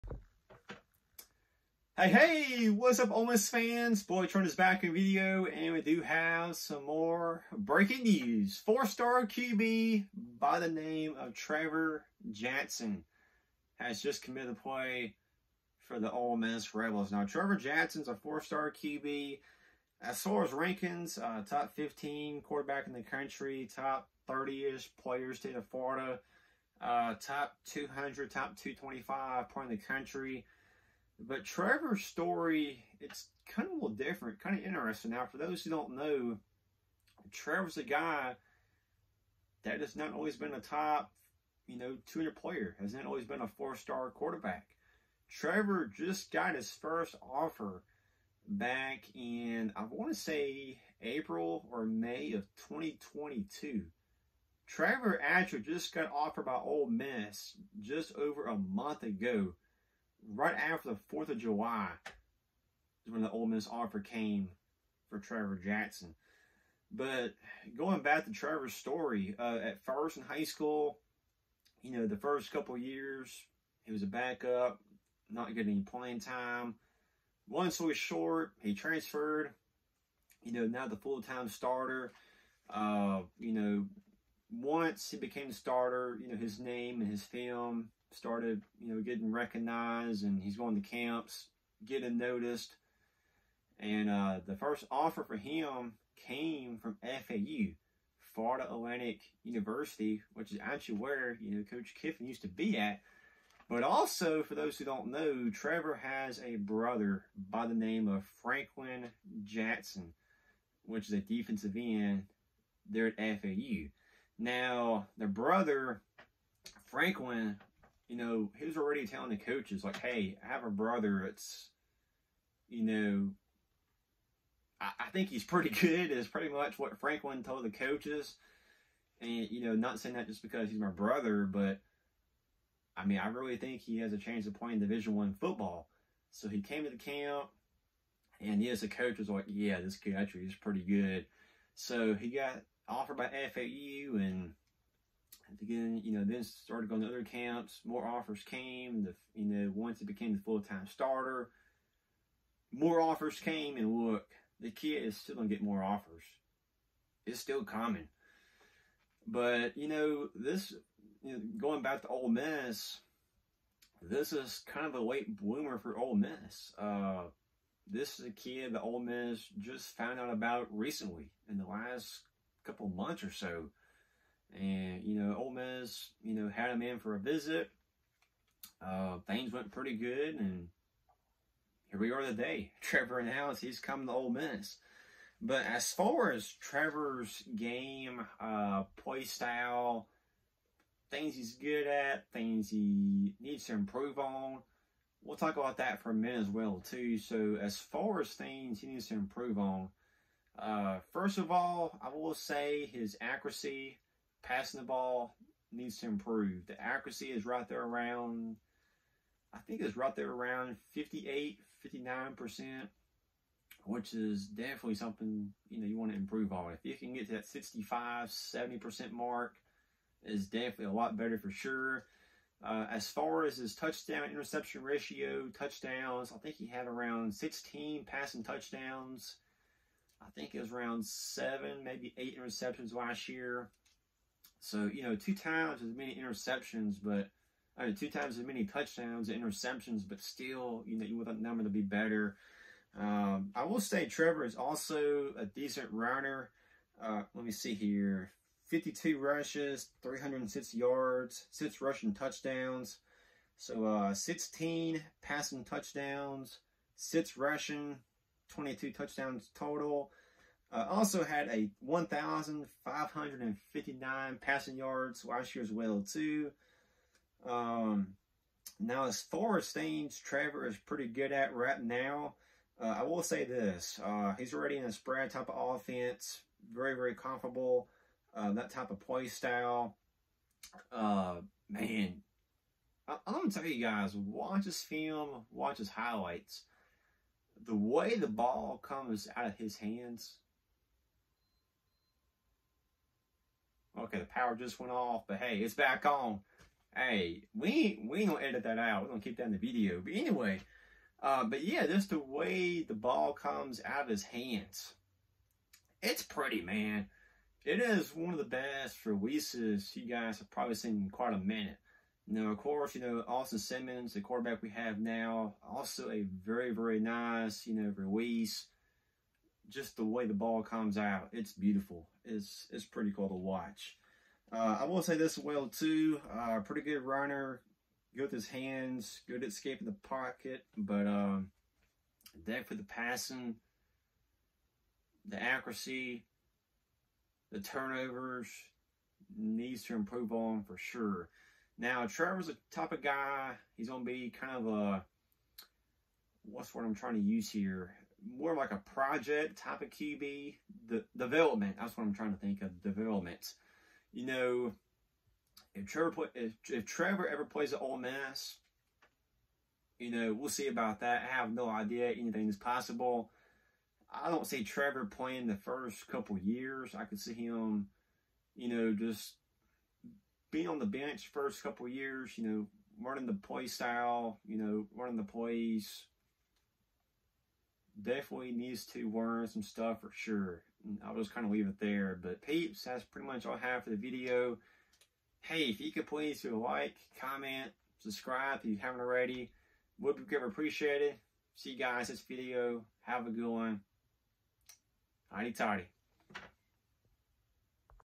4 ⭐️ QB Trever Jackson commits to Ole Miss
Trever Jackson the 4 star QB out of Orlando Florida has just made his college announcement and has choose to Play for the Ole Miss Rebels

In todays video we break down Trevor’s game and talk about what Ole Miss is getting with the 4 star QB Trevor Jackson